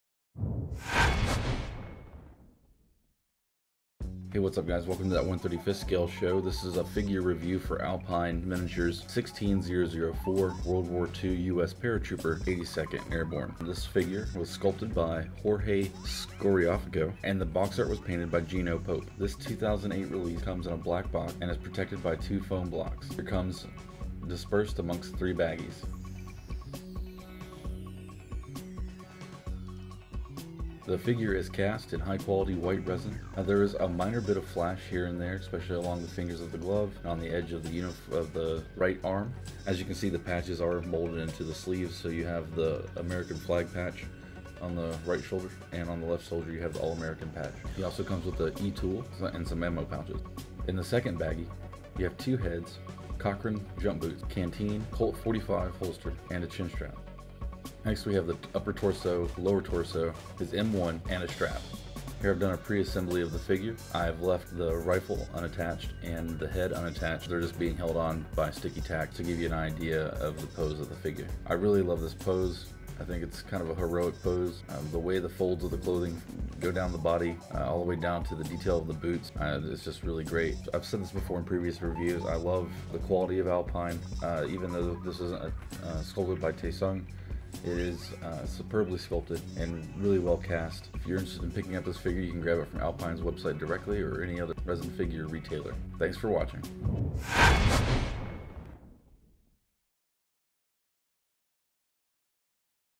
Figure Review: Alpine Miniatures 16004
In this video we check out Alpine Miniatures US Paratrooper 82nd Airborne (All American)  The Figure was sculpted by Jorge Scoriaffico and Box Art by Gino Poppe.